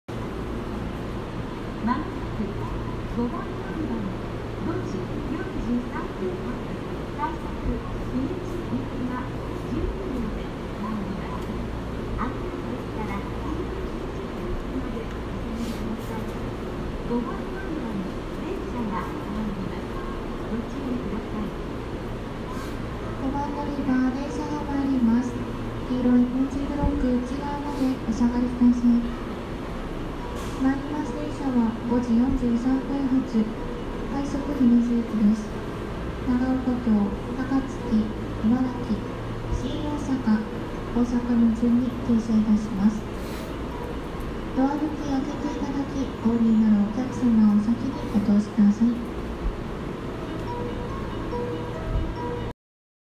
【始発から朝ラッシュ時にかけてのみ運転】快速姫路行き 接近放送
2018年8月11日（土・祝）京都駅にて収録

高槻〜西明石で快速運転を行う普通ですが、始発から朝ラッシュ時にかけてのみ、京都〜西明石で快速運転を行います。このため「高槻から（orまで快速」ではなく、「京都から（orまで快速）」となります。

かつてはほぼ終日運転でしたが、ダイヤ改正を重ねるにつれて運転時間帯は縮小し、始発から朝ラッシュ時にかけてのみの運転となりました。

また早朝は新快速が運転されていないor本数が少ないため、快速が終点まで先着という場合も数多くあります。

京都〜高槻では途中、長岡京のみしか停まらないため、同区間の新快速との所要時間は数分程度しか変わりません。